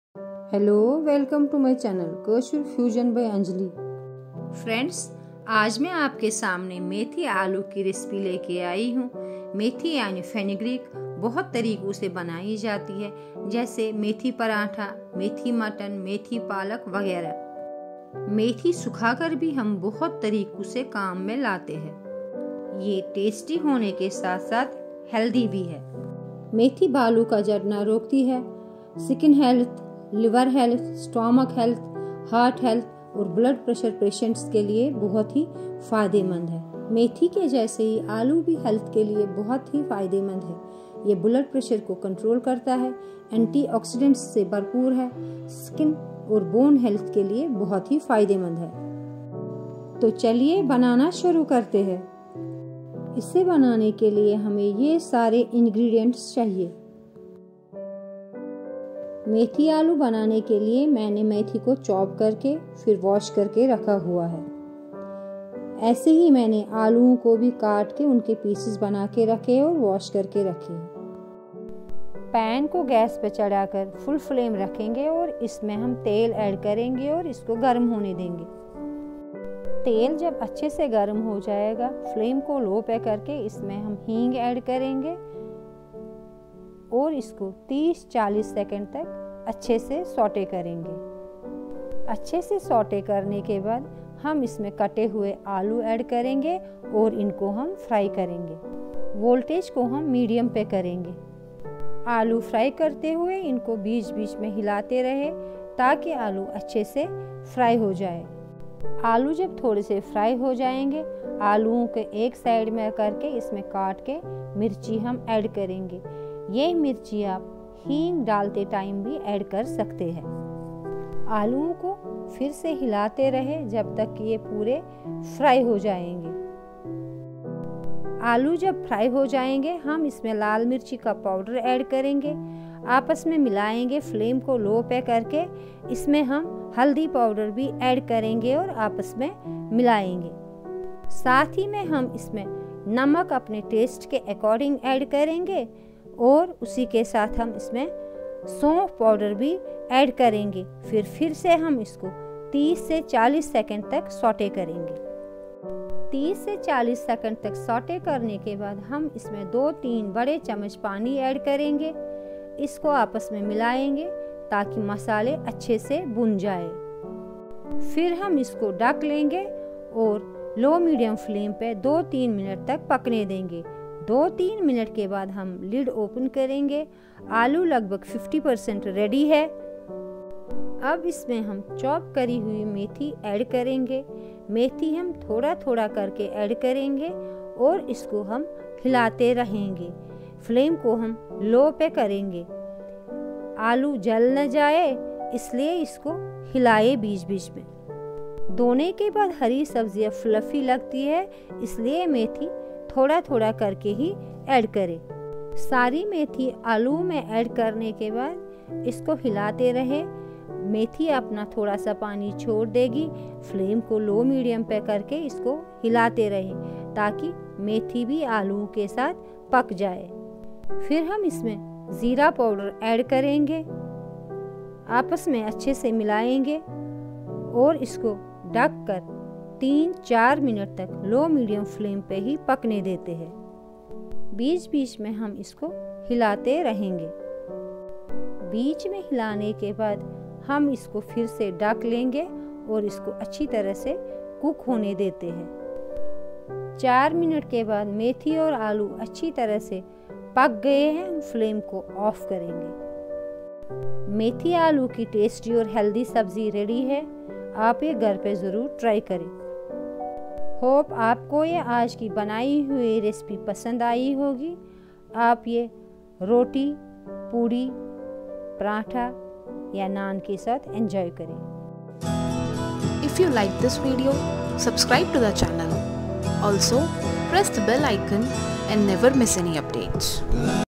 How to cook Aloo-Methi Sabzi Potato-Fenugreek Recipe |आलू मेथी की स्वादिष्ट सब्जी
Potato Fenugreek ( Aloo-Methi) is a yummy and healthy side dish. It is very simple and easy to cook and is slightly spiced. When the mild bitterness of fenugreek and fried potatoes mix together , it gives this recipe an awesome texture and taste .

Happy Cooking.